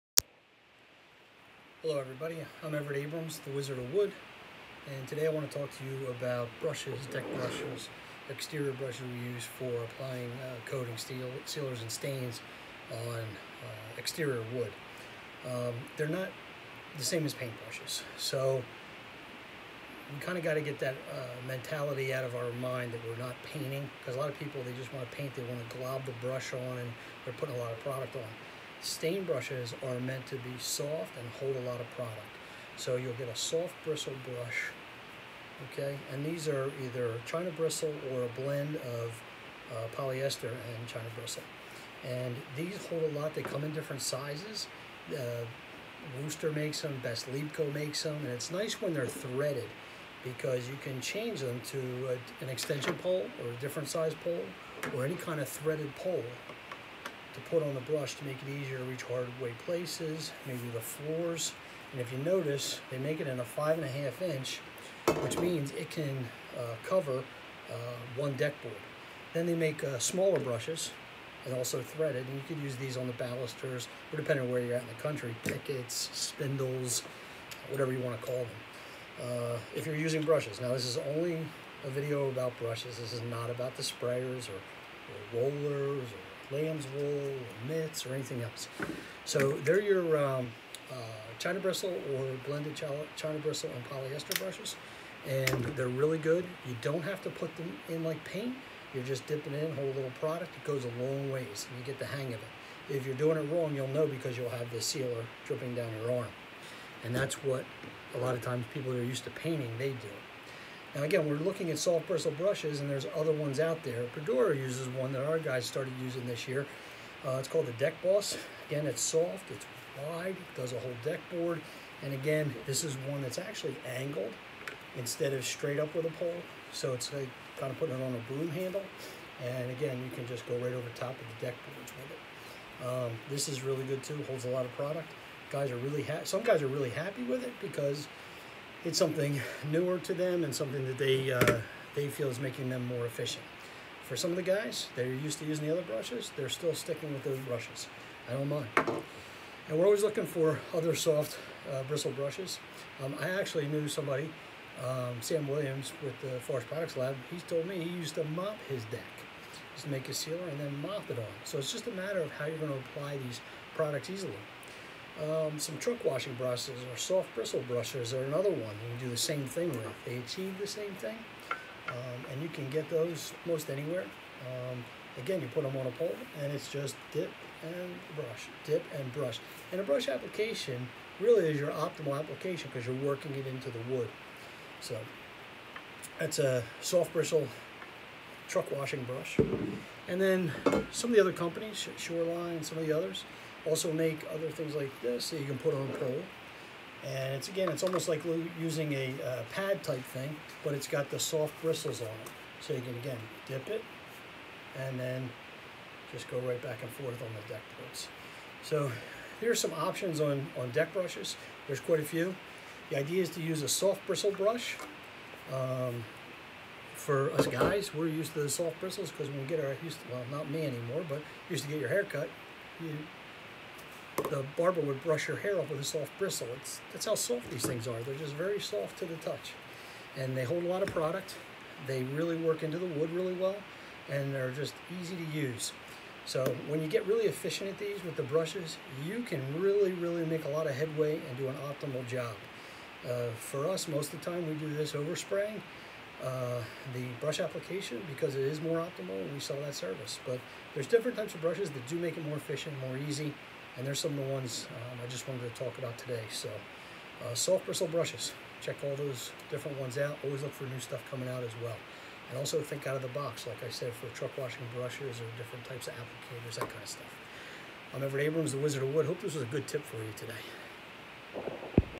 Brushes for Exterior Sealers and Stains by the “Wizard of Wood” Everett Abrams
In this video the “Wizard of Wood” Everett Abrams discusses the different types of brushes that can be used when applying decks sealers and deck stains. This video only discusses brushes as an application method.